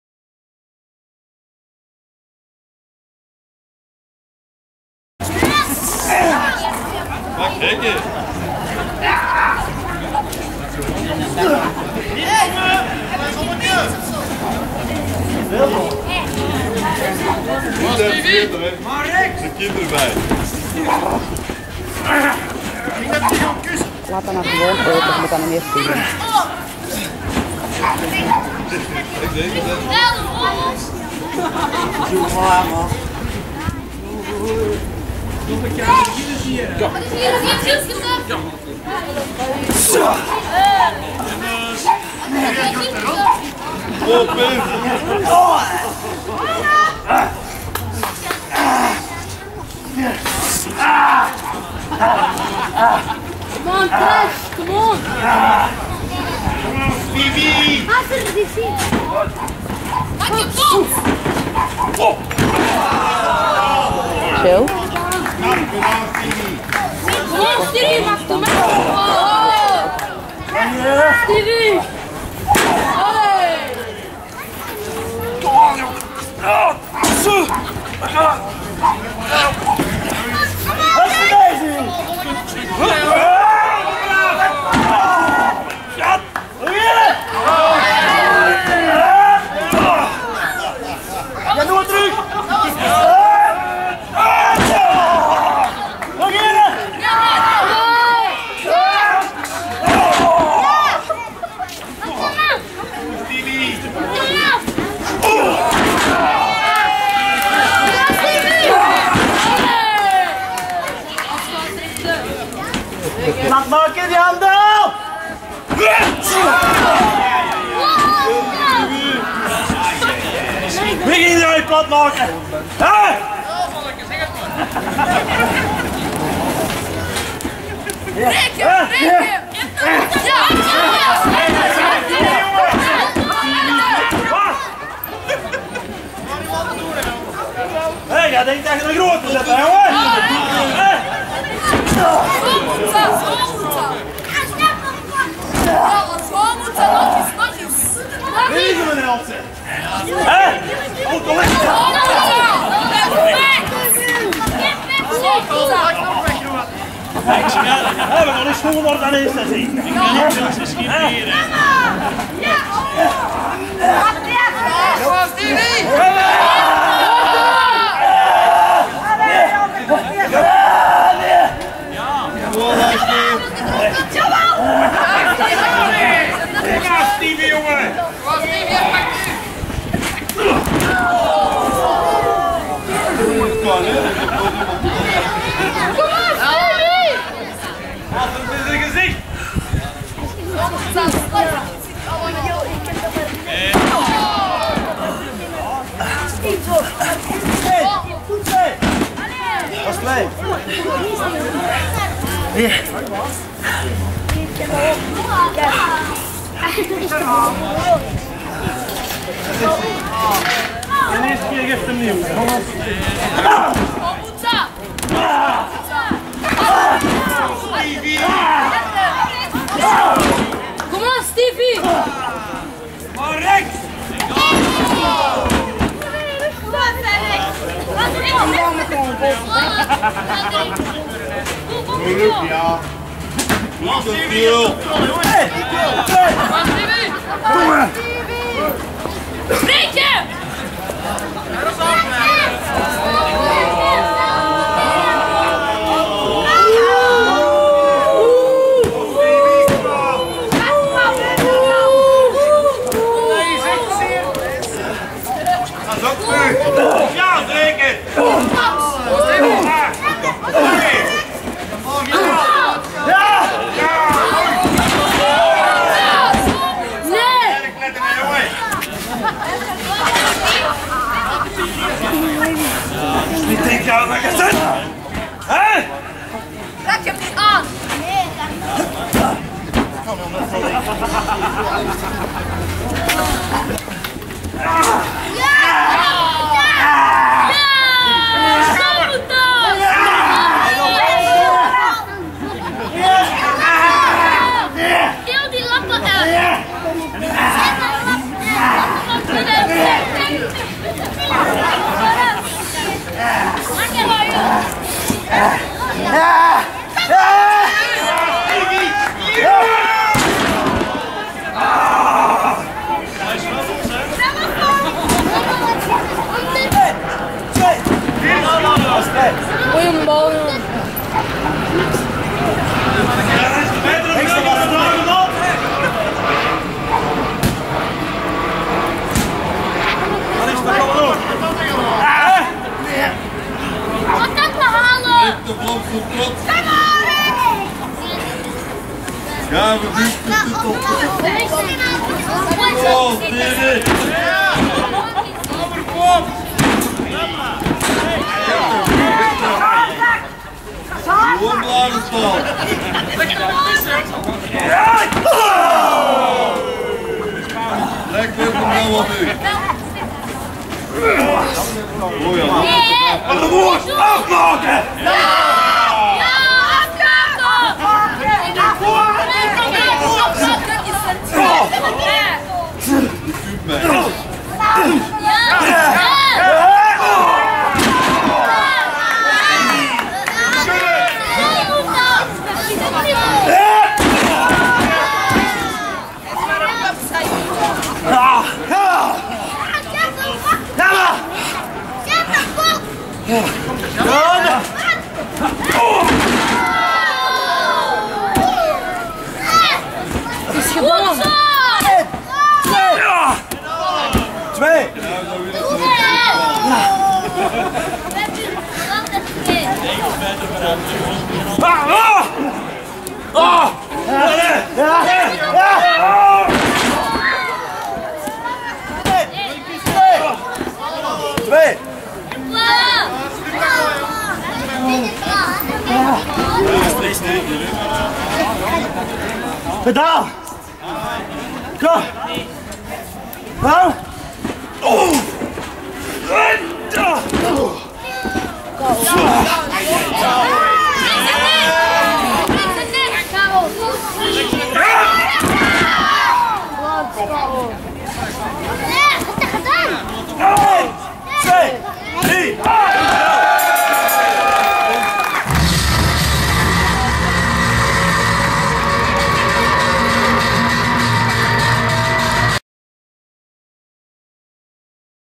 Rex Rage vs Stevie Trash @ FWF on Tour Antwerpen
Rex Rage vs Stevie Trash @ FWF on Tour Antwerpen 27 juli 2014 in de halve finale van het Flemish Championship toernooi